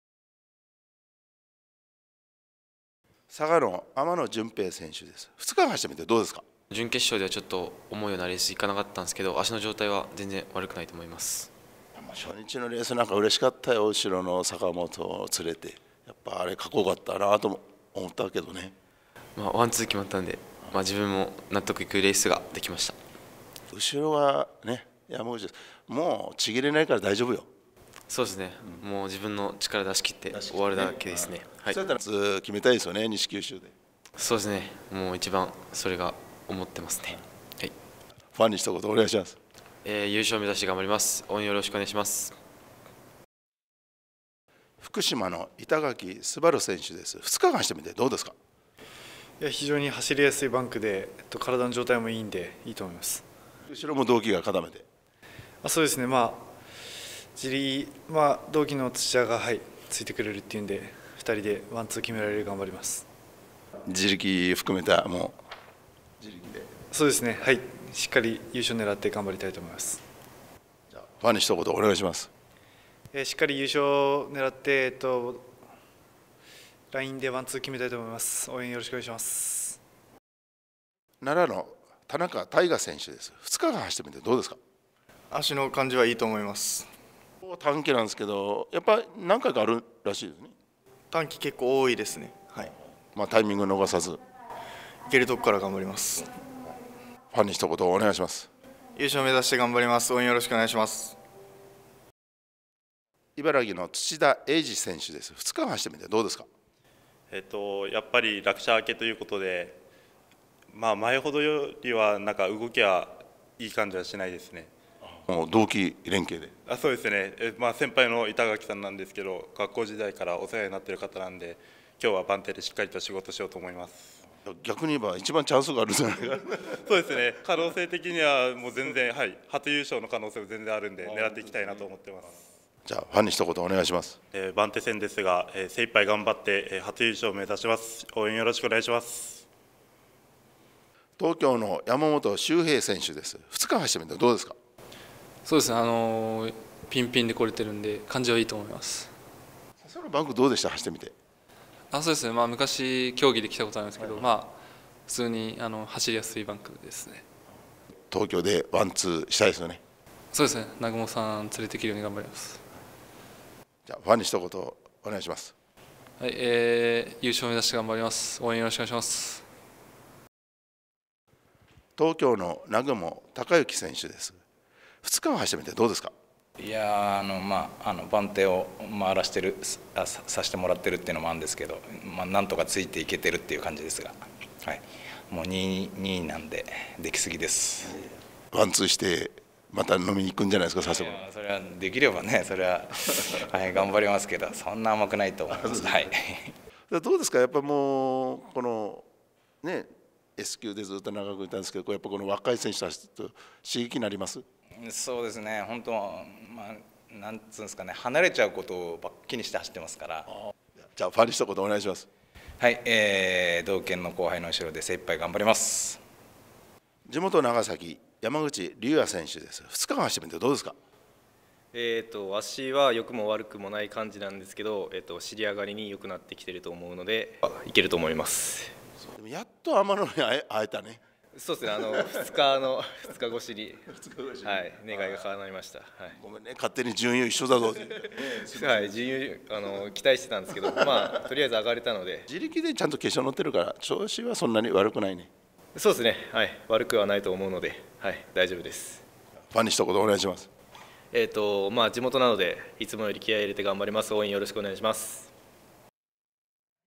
2月6日 佐世保F2 ミッドナイト競輪 7R A級チャレンジ決勝メンバーインタビュー
佐世保ミッドナイト7R決勝メンバー
①天野　純平
②板垣　昴
③田中　大我
④土田　栄二
⑤山本　修平
⑥南雲　孝之
⑦山口　龍也